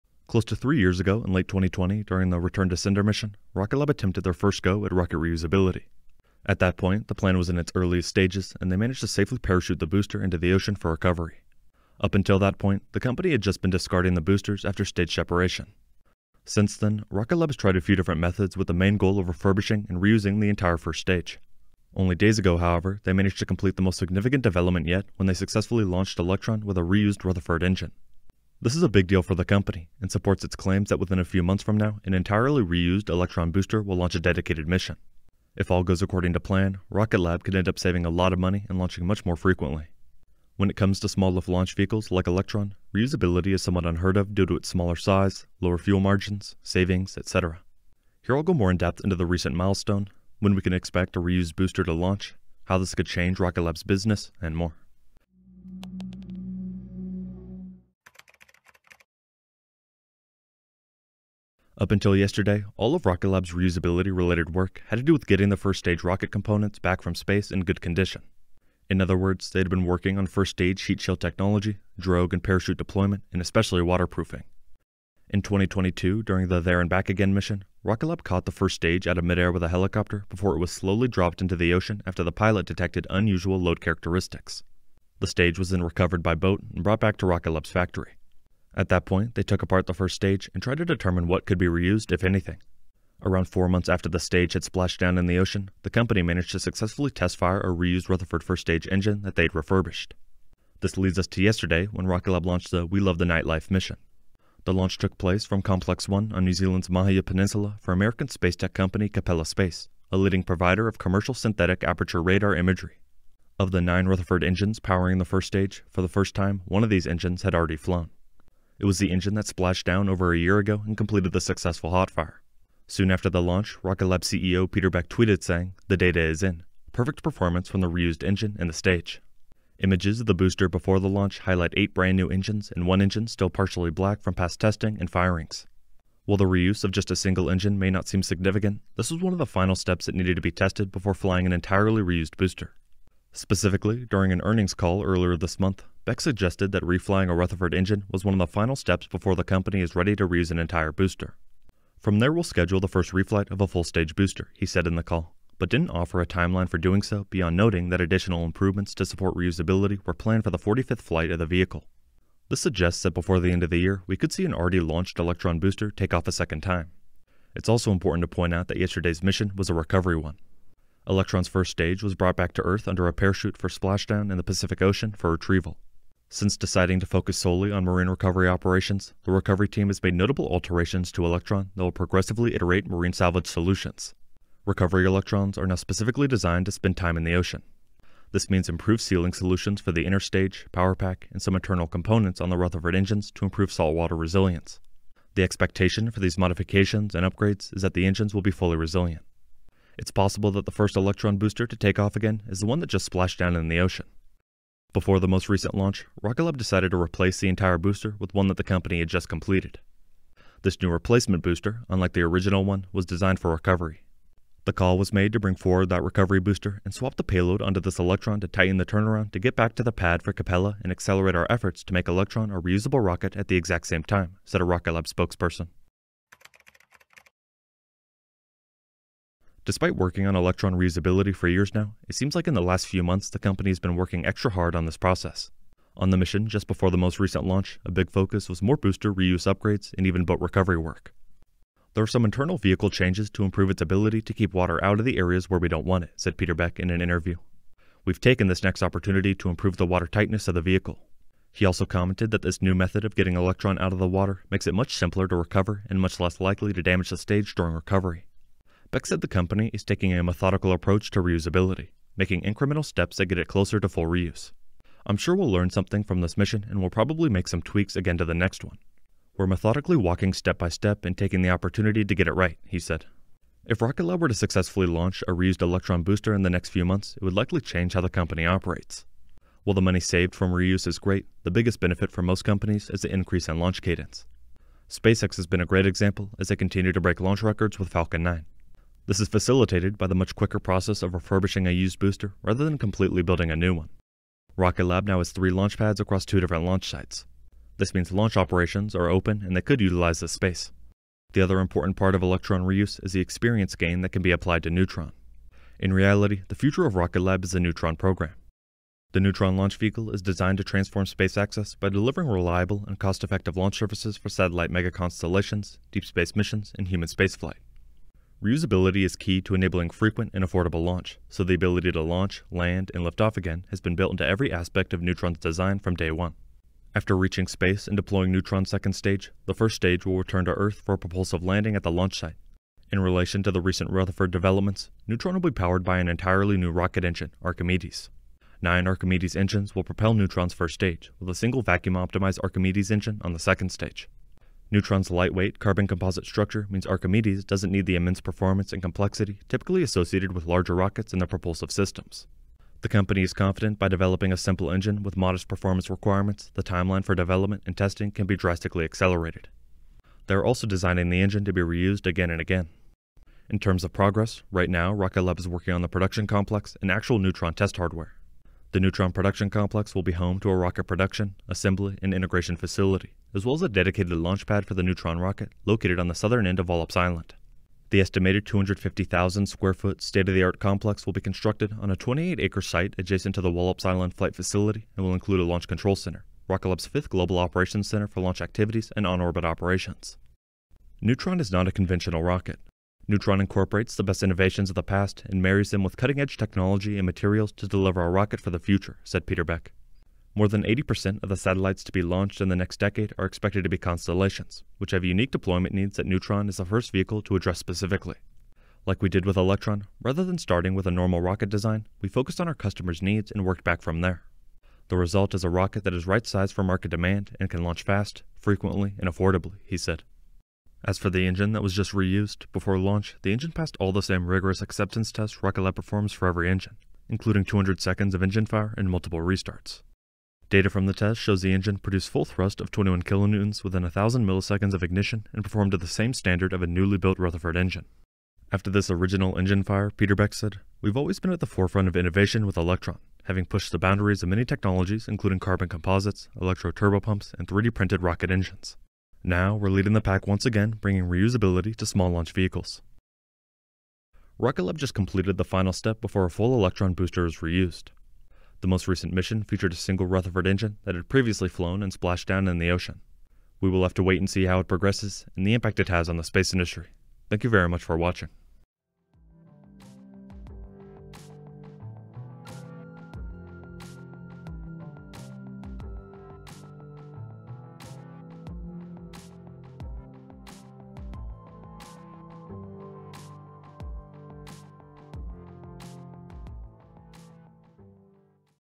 Rocket Lab Just Launched It's First Reused Engine
Close to 3 years ago in late 2020, during the Return To Sender mission, Rocket Lab attempted their first go at rocket reusabilty. At that point, the plan was in its earliest stages and they managed to safely parachute the booster into the ocean for recovery. Up until that point the company had just been discarded the boosters after stage separation.

Since then, Rocket Lab has tried a few different methods with the main goal of refurbishing and reusing the entire first stage. Only days ago however they managed to complete the most significant development yet when they successfully launched Electron with a reused Rutherford engine. This is a big deal for the company and supports its claims that within a few months from now, an entirely reused Electron booster will launch a dedicated mission.

If all goes according to plan, Rocket Lab could end up saving a lot of money and launching much more frequently. When it comes to small lift launch vehicles like Electron, reusability is somewhat unheard of due to its smaller size, lower fuel margins, savings, etc. Here I will go more in-depth into the recent milestone, when we can expect a reused booster to launch, how this could change Rocket Lab’s business, and more.

Chapters:
0:00 - Intro
0:58 - First Engine Reuse
3:45 - Waterproofing A Rocket